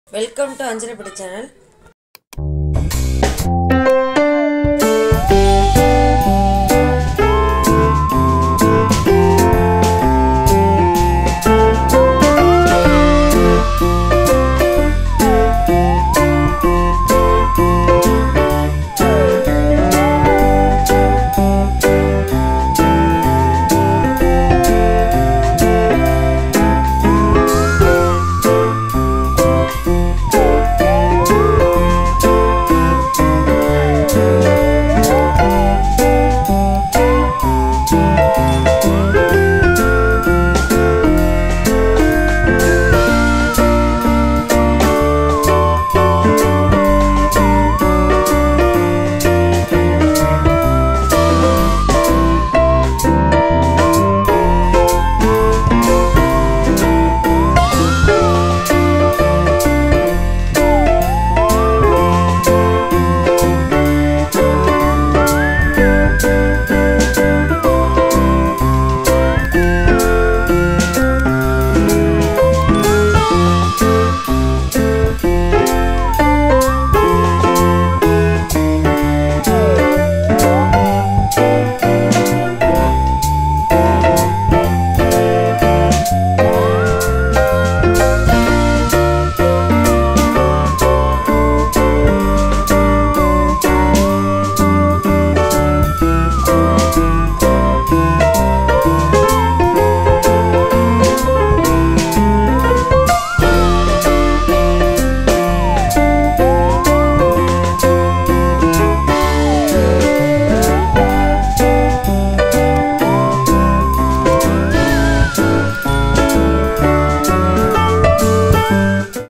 Instant Poha Mix Recipe Teamil Anjara Petti | (Travel-Friendly & Budget-Friendly) Ready to Breakfast
In this video Looking for a quick and easy breakfast option? Check out this Instant Poha Mix Recipe that is not only travel-friendly but also budget-friendly. Start your day right with this delicious and convenient meal option! To please. So why wait? Try it out and let us know how it turns out in the comments below! Happy cooking

Instagram page link And Instagram Follow anjarapetti2023 And All Hand Watch Reel videos please Follow My

Happy cooking With @anjarapetti